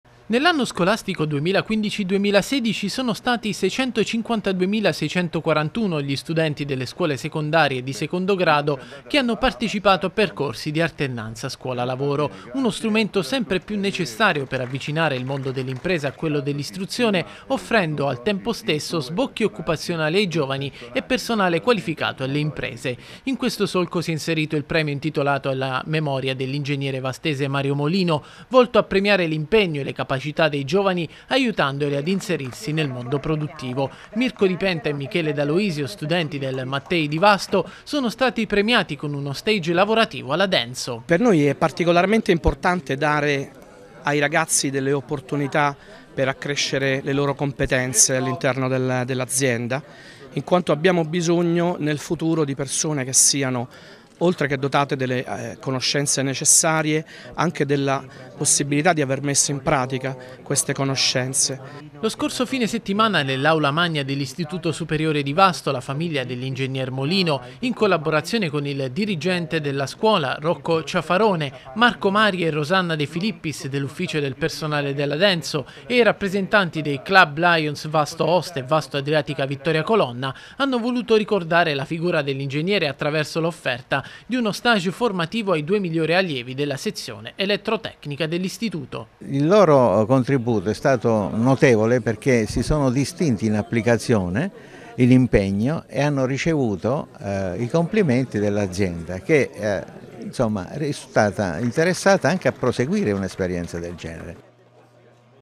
Premio Molino per stage alla Denso di Vasto
L’alternanza scuola lavoro è il primo obiettivo del premio intitolato alla memoria dell’ingegnere vastese Mario Molino che, quest’anno, ha premiato, con un stage alla Denso, due studenti dell’Istituto Mattei